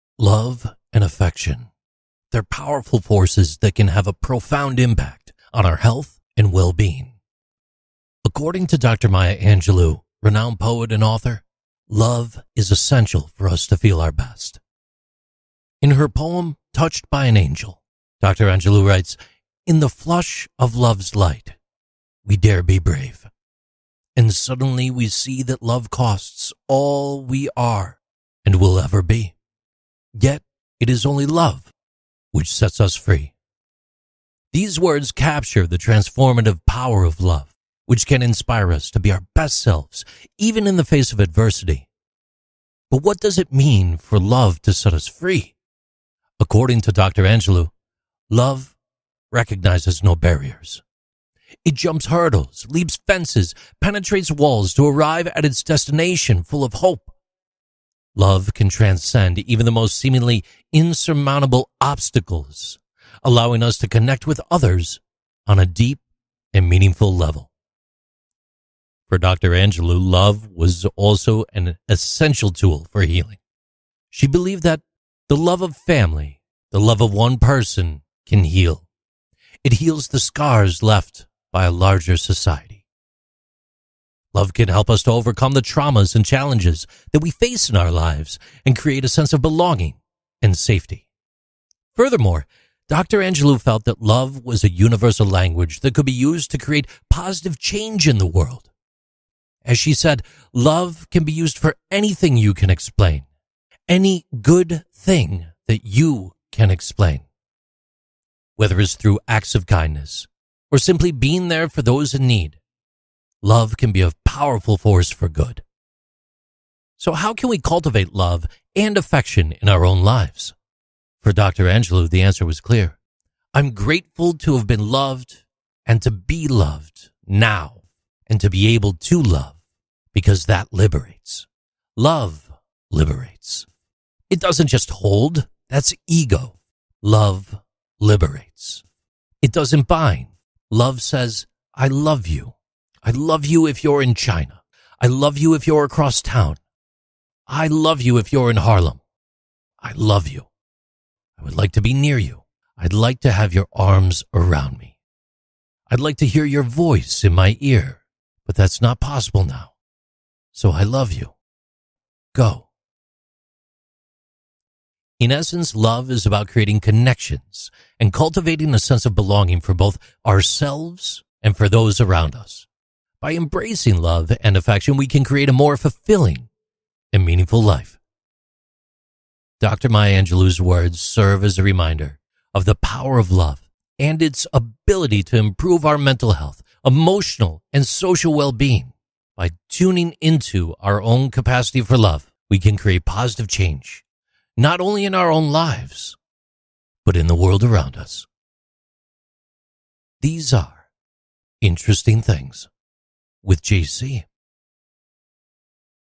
553: "Thoughts of Love, Dr. Maya Angelou, and You"
Interesting Things with JC #553: "Thoughts of Love, Dr. Maya Angelou, and You" - Love has the power to heal and liberate, impacting our mental, emotional and social well-being. Let's reflect on Maya Angelou's wise words and the importance of love in our lives.